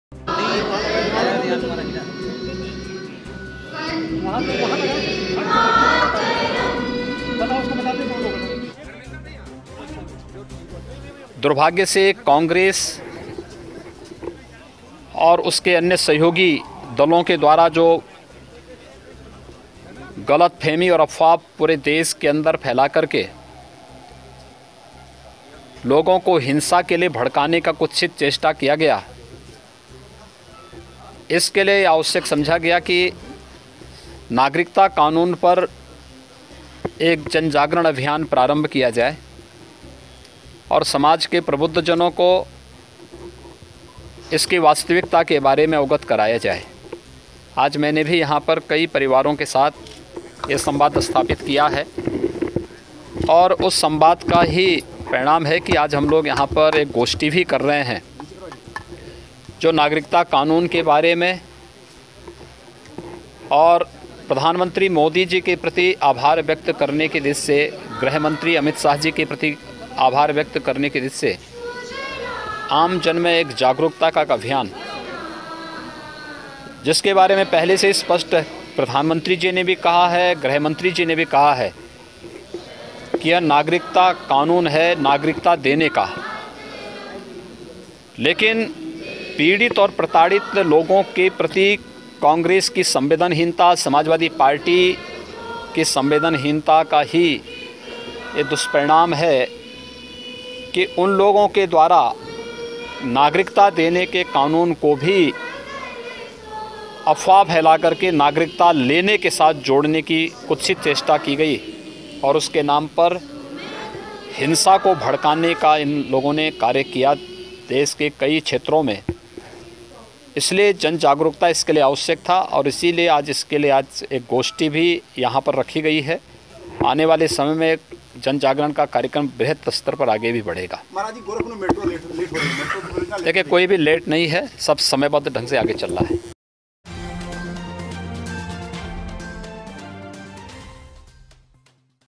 पीड़ित प्रताड़ित लोगों की मदद के लिए है, नया नागरिकता कानून। UPCM Yogi
द इंडियन ओपिनियन की इस खबर में देखिए, पीड़ित प्रताड़ित लोगों की मदद के लिए है, नया नागरिकता कानून। सीएम योगी